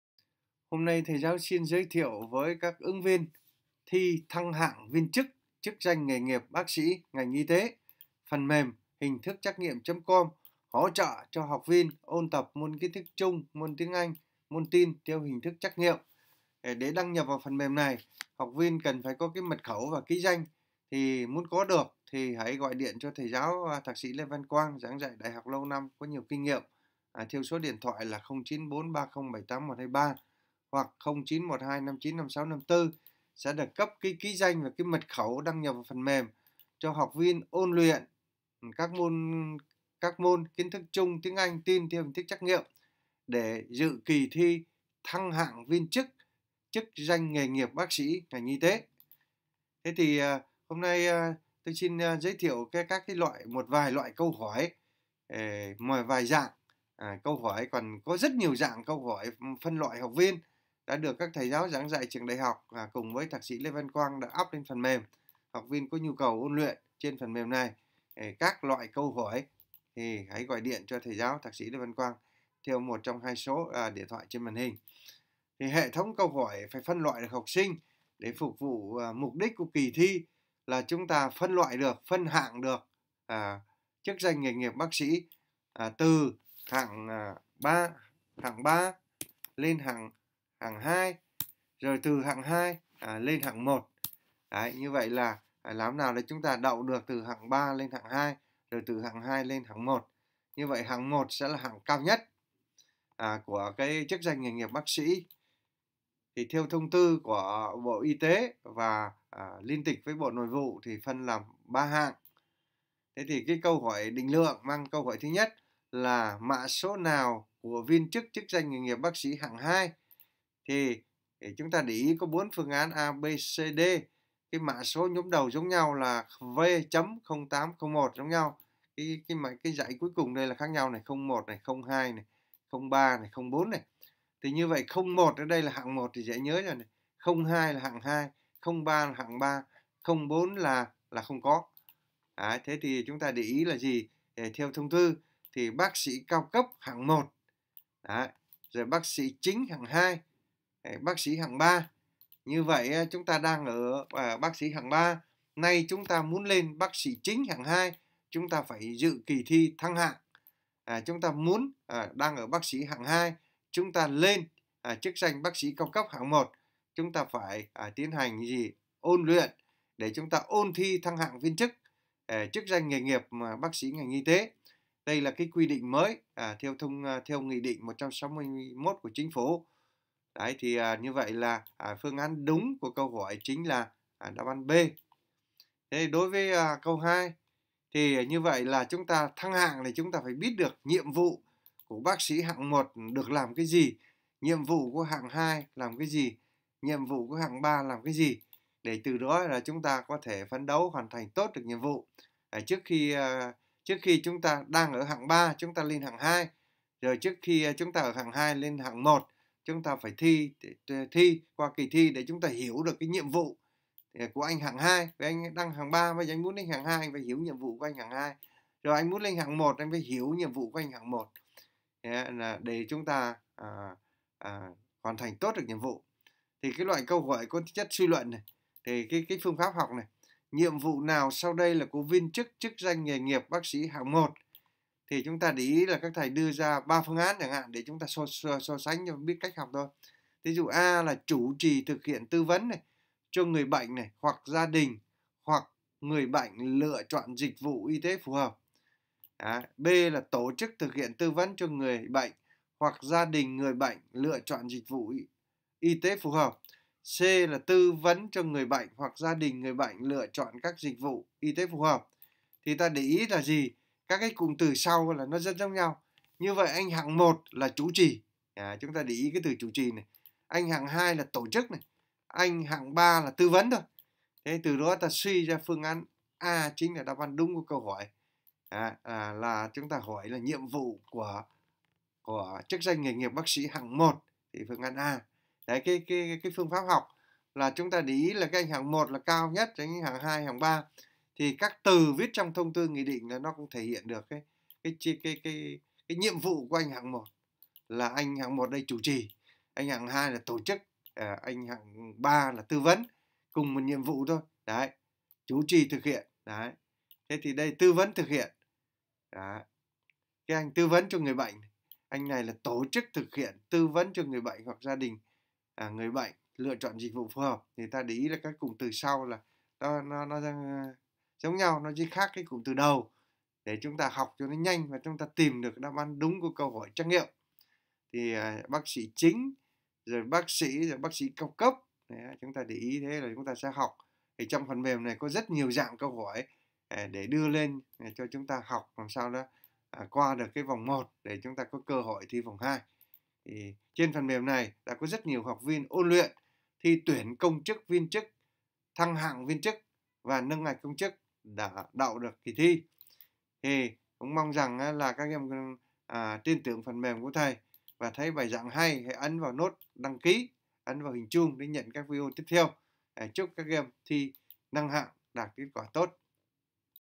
Ôn thi thăng hạng Viên chức CHỨC DANH NGHỀ NGHIỆP BÁC SĨ Y (ngành y tế)
Ôn thi thăng hạng Viên chức
CHỨC DANH NGHỀ NGHIỆP BÁC SĨ  (ngành y tế)
-Câu hỏi trắc nghiệm Môn kiến thức chung, tiếng anh, tin học